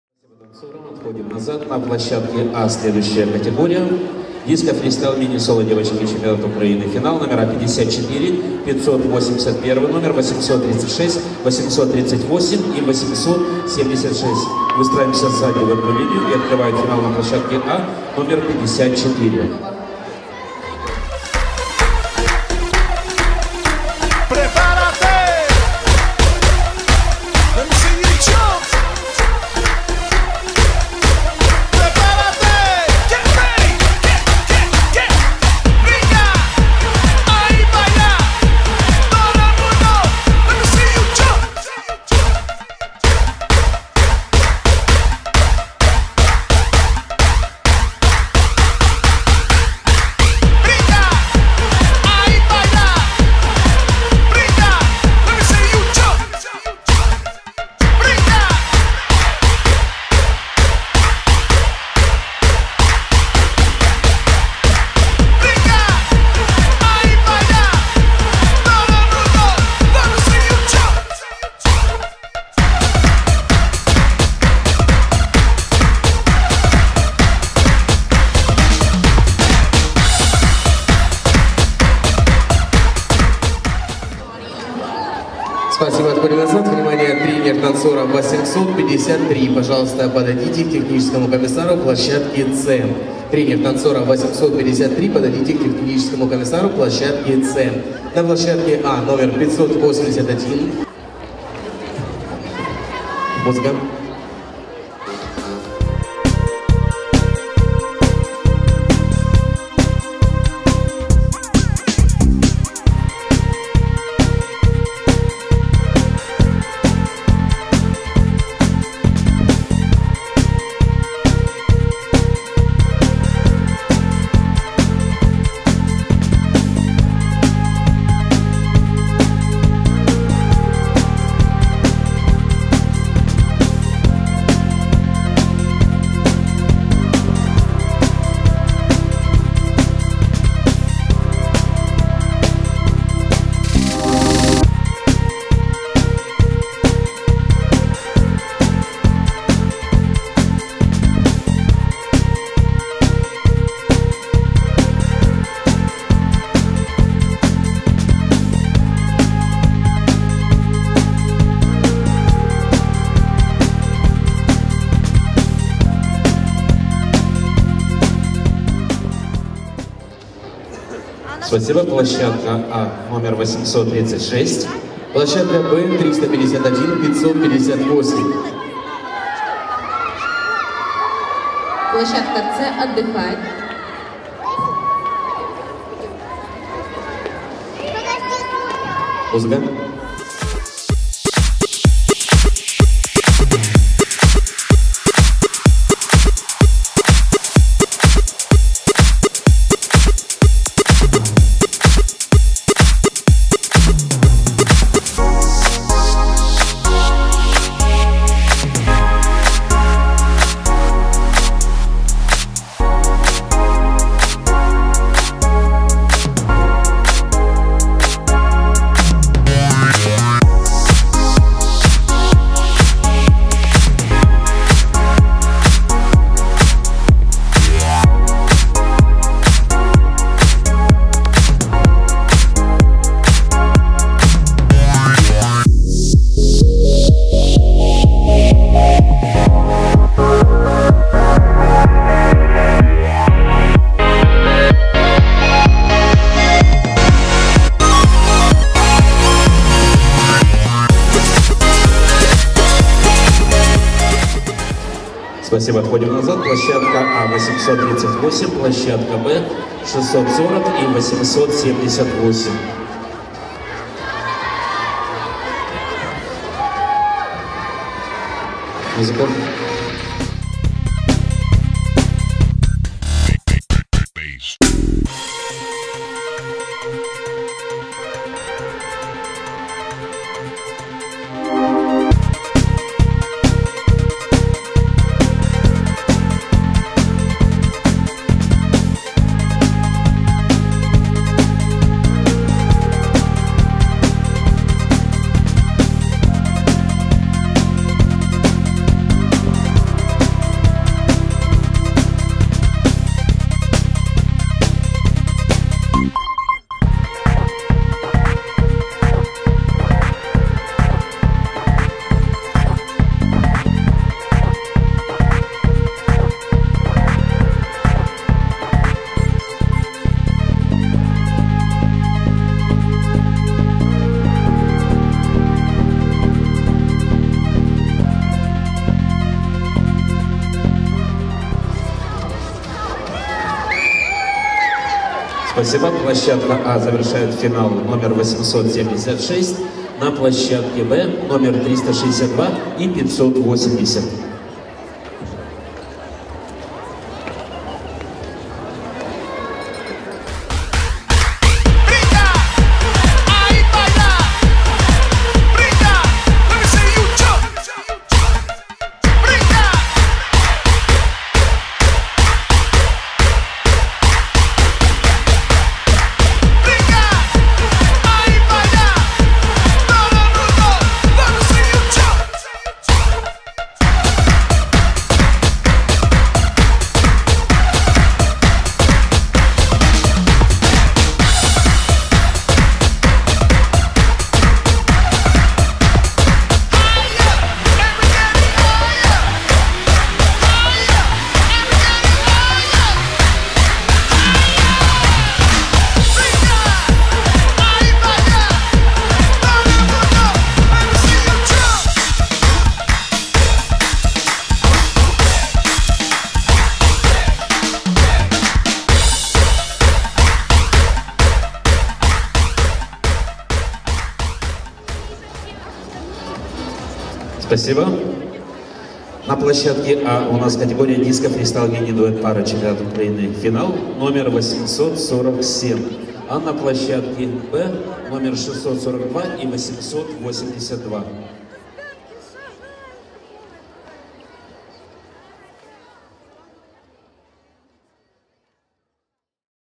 Disco Freestyle FINAL ☀ Mini Solo Girls ☀ Championship of Ukraine
00:20 Saiko Uliana
01:40 Dmytruk Daria
03:10 Olefirenko Sofiya
04:28 Pinkas Oleksandra
05:50 Hlotova Anna-Alina

Area «A». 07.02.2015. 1st Section.

Original sound of performances is replaced because of copyright questions.